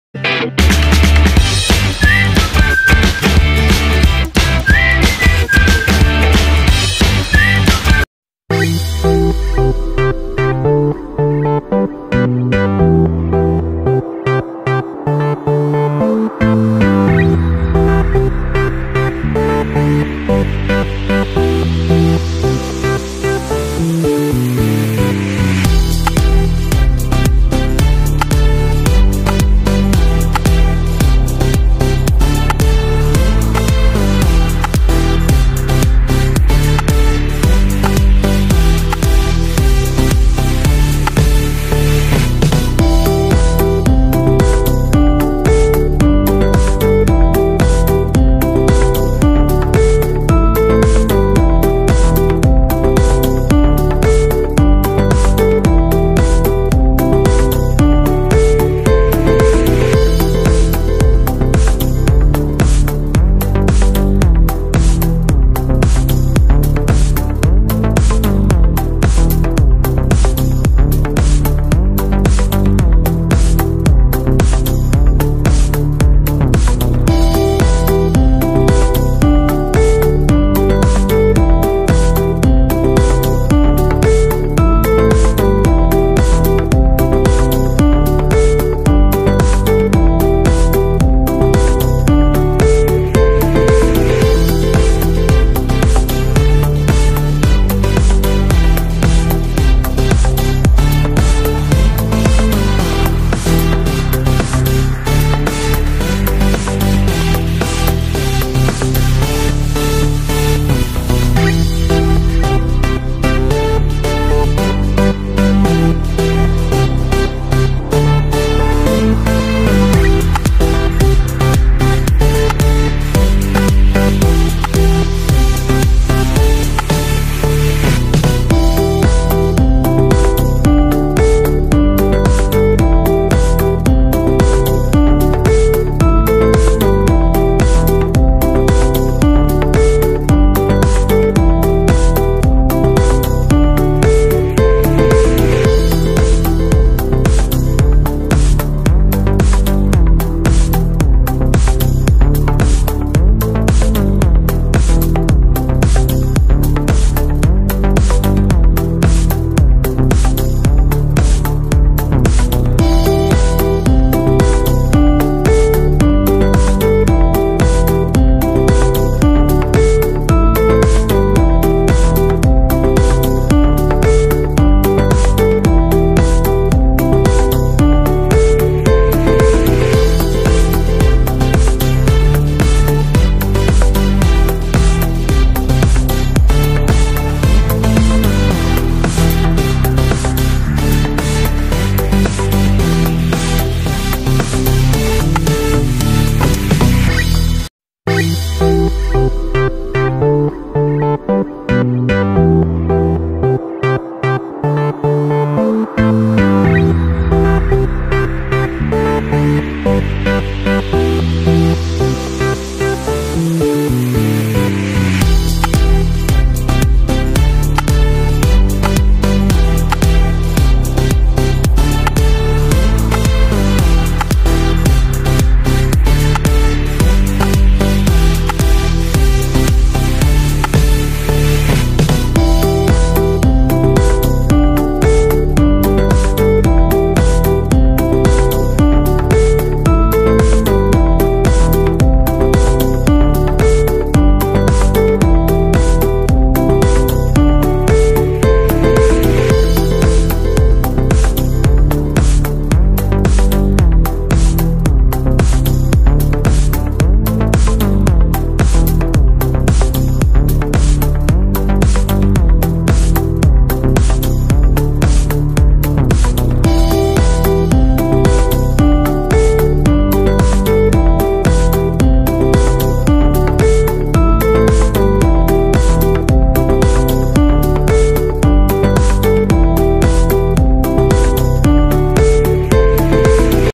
Apple muraba recipe . Seb ka muraba easy recipe
Seb ka bohat hee mazydaar murabba bnany ka asaan tarika ..
easy recipe